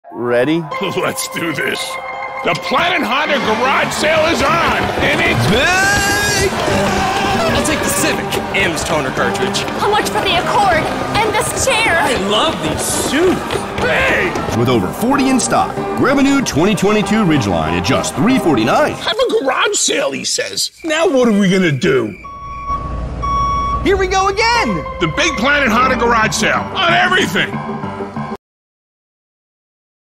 The Planet Honda Garage Sale is ON! Get BIG SAVINGS in a new Ridgeline
Planet Honda the place to be to get a new Honda Ridgeline, with over 40 in stock! Grab a new 2022 Ridgeline RTL at just $349 during our Garage Sale at Planet Honda NH!

Planet Honda is one of the newest Honda Dealerships in New Hampshire! We're excited to meet you and serve you.

Follow us for more savings!